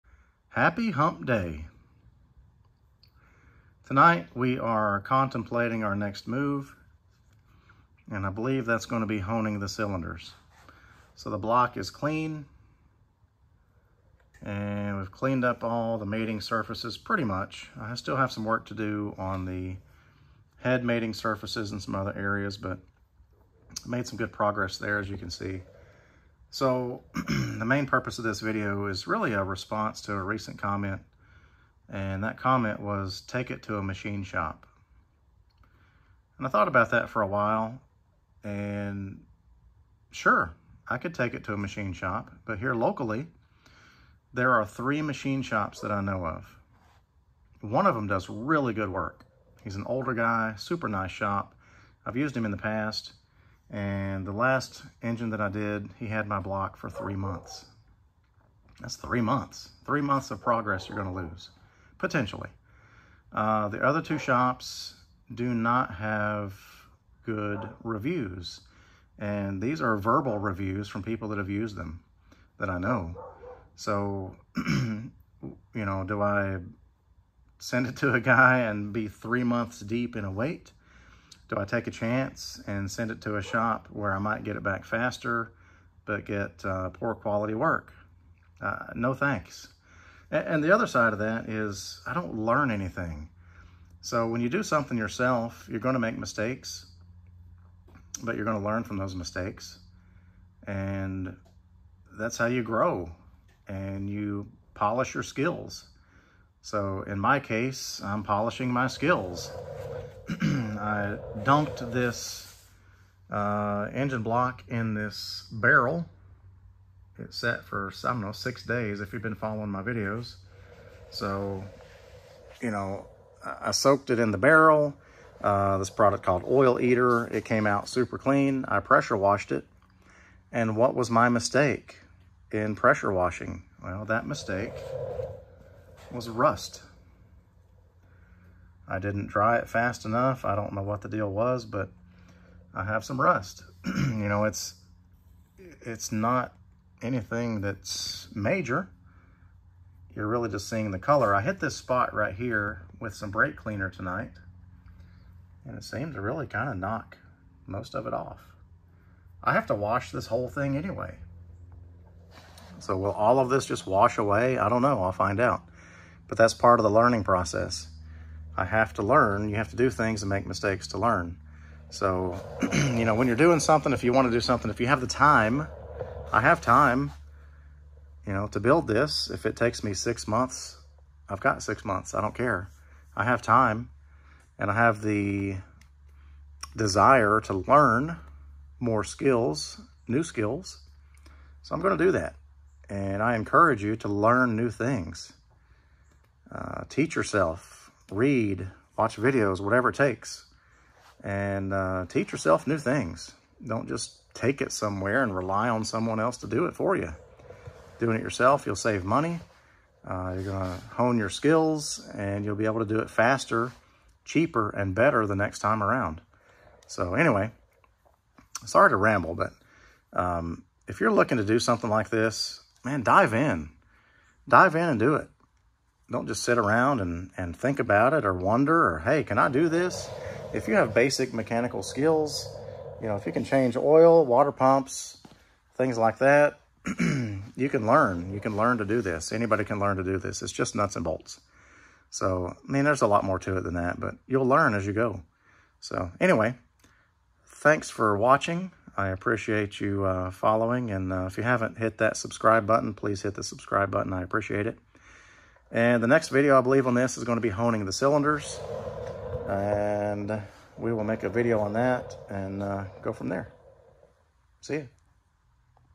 "Take it to a machine shop" they said. No thanks, I can do this myself.
I don't really know how to describe this one. If you have the time and desire to do something, LEARN how to do it! We live in a time where there is SO MUCH information available and at our fingertips that there is no reason you can't learn how to do ANYTHING. I just turned 51 and at this point in life, I'm HUNGRY to learn as much as I can and be as SELF SUFFICIENT as I can be. This is not only applicable to mechanical things (which I excel at) but other things as well regarding general life and sustainability.

STOP relying on others to do things for you and dive into life! Save money, learn new skills and really LIVE! I hope this video inspires you to take on that project and finish it with a huge SUCCESS!